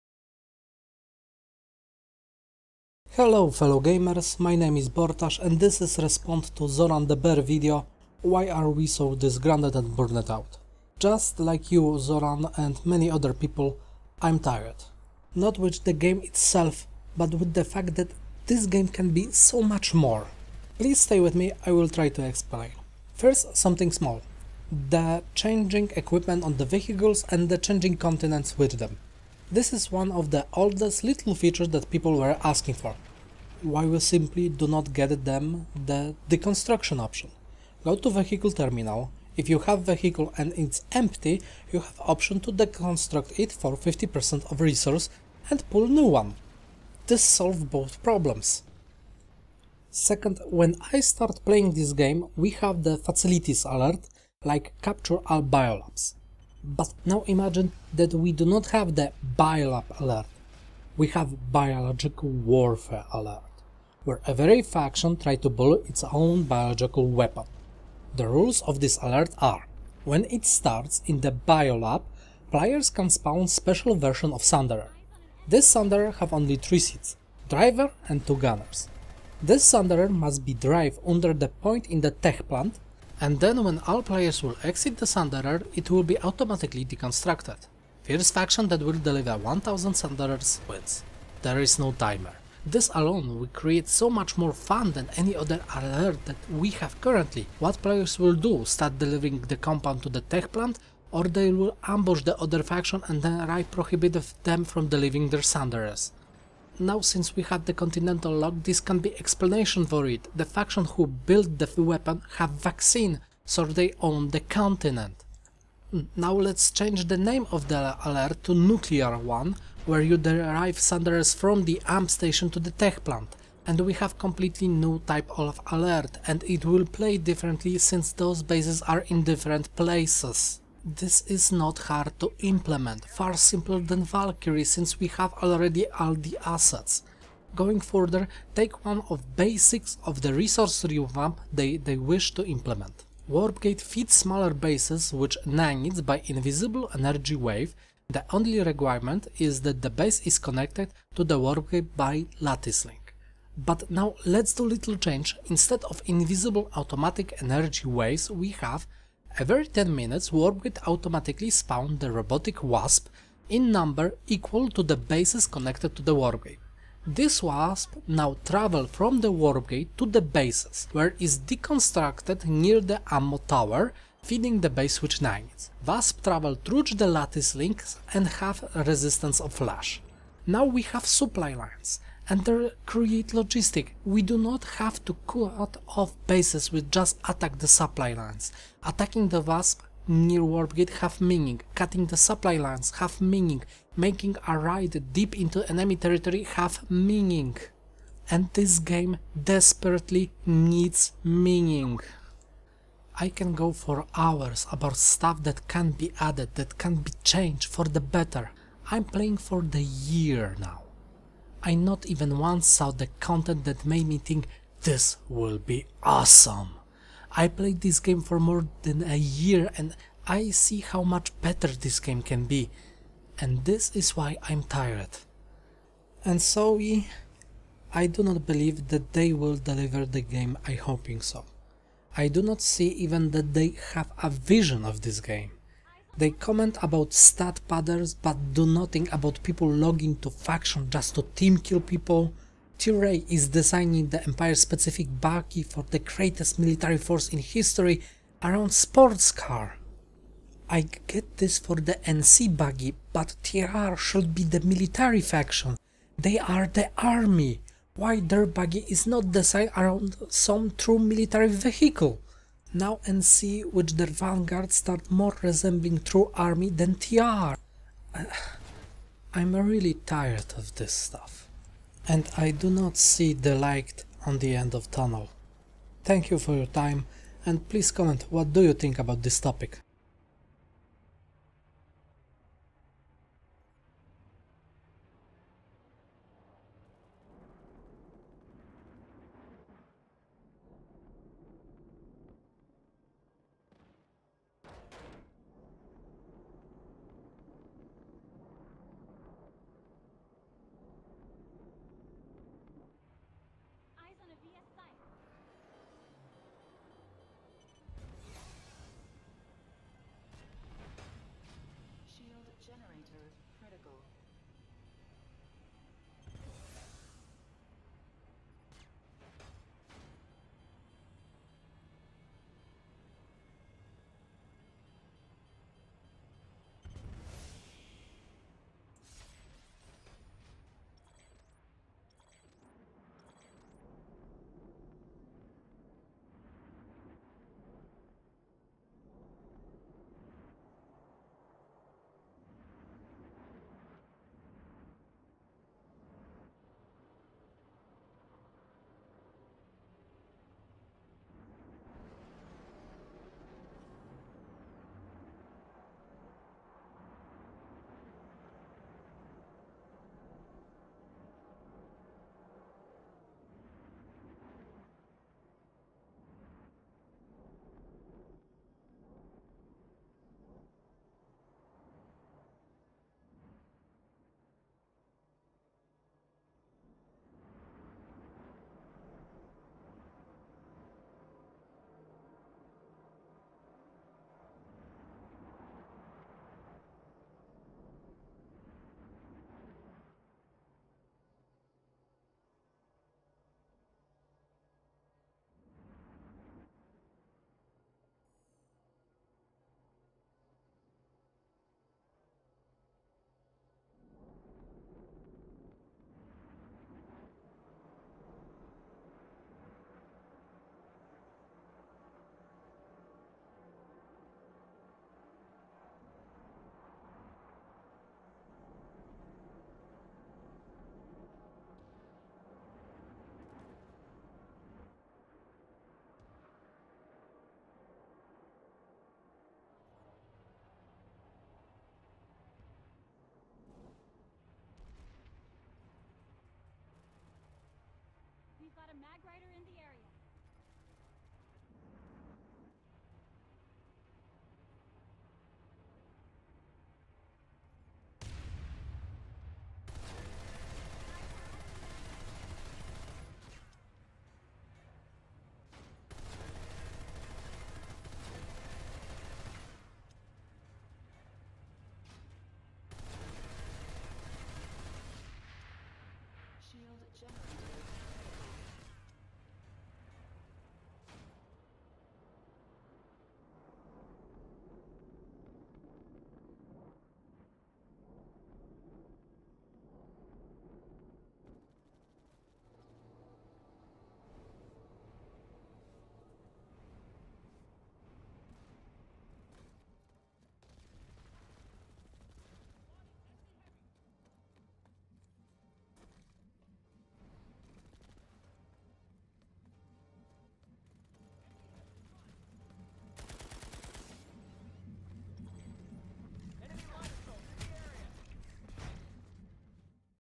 Re: Planetside 2 Real Talk ► Why are we so disgruntled and burned out?
Hello fellow gamers.
My name is Bortasz and this is respond to ZoranTheBear Video: Why are we so disgruntled and burned out?

Just like Zoran and many other people I’m tired. Not with the game it self, but with the fact that this game can be so much more.
Pleas stay with me I will try explain.

First something small. The changing equipment on the vehicles and the changing the continents with them. This is one of the oldest little features that people were asking for.
Why we simple do not get the deconstruction option? Go to vehicle therminal, if you have vehicle and it’s empty you have option to deconstruct it for 50% of resources and pull new one. This solve both problems.

Second when I start playing this game, we have the facilitie alert like capture all bio labs.
But now imagine that we do not have the bio lab alert. We have biological warfare alert. Were every faction try to build it’s own biological weapon.
The rules of this alert are:
- When it starts in the Bio lab(Factory that put together the weapon), players can spawn special version of sunderer.
- This sunderer have only 3 seats. Driver and two gunners. (The rest of place is reserve for the biological weapon)
- This sunderer must be drive under the point in the Tech plant (Place that will disperse the virus)
- And then when all players will exit the sunderer it will be automaticly deconstructed.
- First faction that will deliver 1000 sunderers wins.
- There is no timer.
This alone will create so much more fun that any other alert that we have currently.
What players will do?
Start delivering the compound to the tech plant? Or they will ambush the other faction and try prohibit them from delivering there sunderers?
Now since we have the continental lock this can be explanation for it. The faction who build the weapon have vaccine.
Now let’s change the name of the alert to the Nuclear were you drive sunderers from the Amp station to the Tech plant. And we have completely new type of alert. And it will play differently since thoes bases are in different places.
This is not hard to implement. Far simpler than valkyrie since we have alredy all the assets.

Going further. Take one of basic of the resource revamp that they wish to implement.
- Warpgate feed smaller basses with nanites by invisible energy waves. The only requirment is that the base is connected to the warpgate by the lattice links.
But now let’s do little change. Instead of invisible automatic energy waves we have:
- Every 10 minutes warpgate automaticly spawn the robotic wasp in number equal to the bases connected to the warpgate.
- This wasp now travel from the warpgate to the basses were is deconstructed neer the ammo tower feeding the base with nanites.
- Wasp travel through the lattice link and have resistancee of flash.
Now we have suply lines. And they create logistics. We do not have to cut off the basses we just attack the suply lines. Attacking the wasp near warpgate have meaning. Cutting the suply lines have meaning. making a ride deep in to the enemy terrytory have meaning.

And this game desperetly needs meaning.

I can go for hours about stuff that can be added, that can be change for the better.
I’m playing this game for more than a year. I not even once saw the content that make me think
This will be awesome.” I play this game for more than a year and I see how much better this game can be. And this is why I’m tired.

And SOE. I do not believe that they will deliver the game I hoping so. I do not see even that they have a vission of this game.
They comment about stat padders, but do nothing about people login to faction just to team kill people.
TRay is designing the empire specific bugy for the greatest military force  in history around sports car.
I get this for the NC bugy, but TR should be the Military faction. They are the army. Why there buggy is not design around some true military vehicle.
Now NC with there vanguard start more resembling true army than TR. I’m realy tired of this stuff. And I do not see the light on the end of tunnel.

And pleas comment what do you think about this topic.